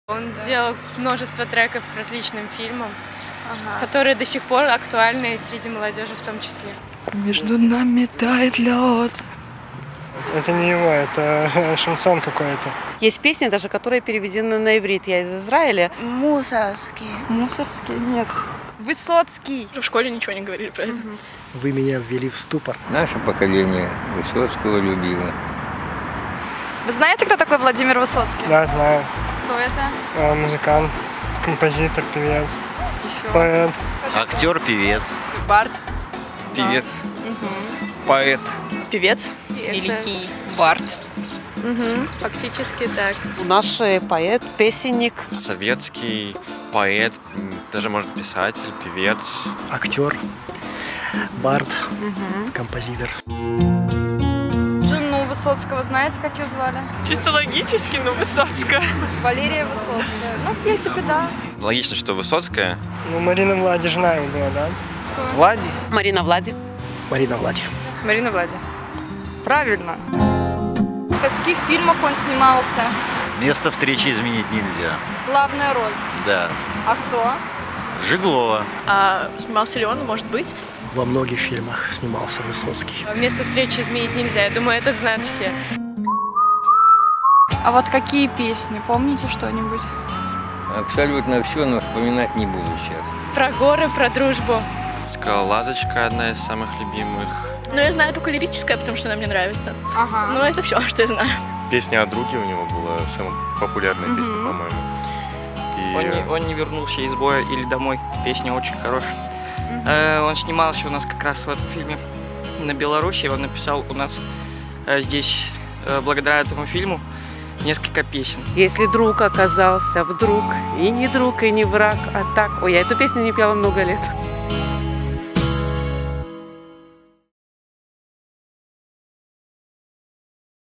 Почему молодежь не знает ? "Кто такой Высоцкий"
Мы решили проверить,  а знает ли молодежь знаменитого поэта, актера и музыканта, его творчество и биографию.
Проверьте себя и честно скажите – знали ли вы ответы на все вопросы, которые задавались ребятам.